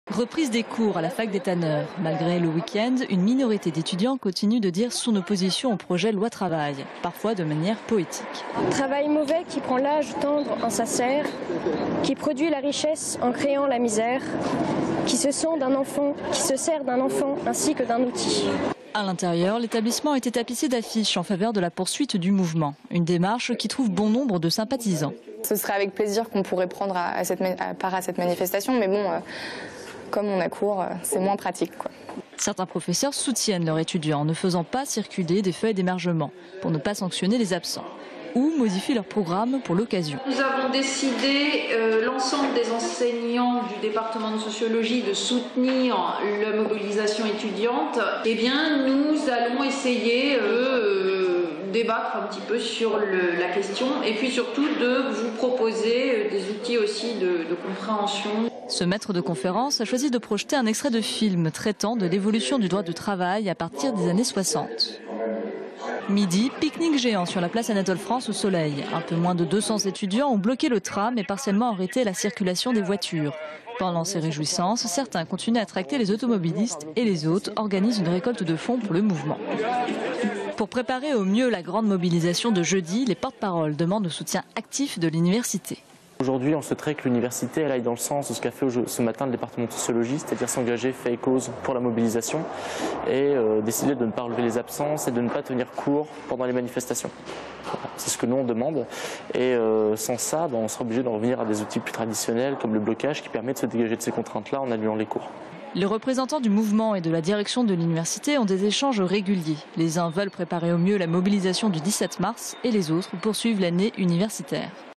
Loi-travail: Fac des tanneurs bastion de la contestation
Sites webs/blogs, copiez cette vidéo avec le bouton "Intégrer" !
 Près de 200 étudiants se sont rassemblés place Anatole France pour protester contre le projet de loi El Khomri. Un mur d'expression a été installé sur le parvis de l'université des tanneurs.